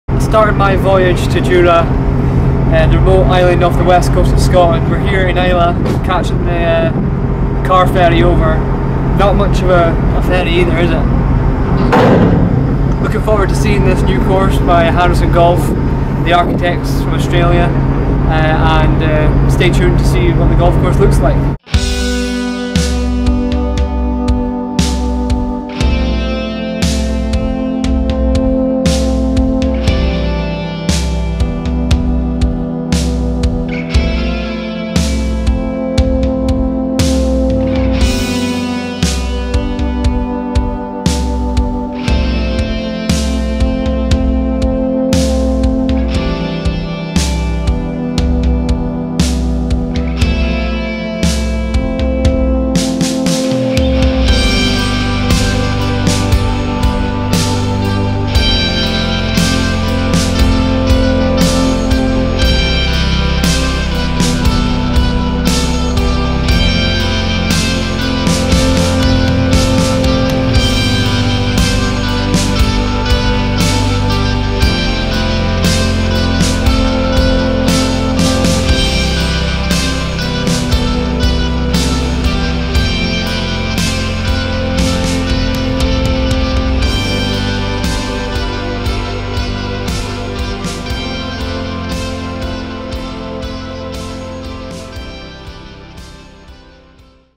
Ardfin Golf Course on Jura is Spectacular!
Ru Macdonald takes a tour of the new golf course under construction on the Isle of Jura, in Scotland's southwest. The course is due to be completed Summer 2017.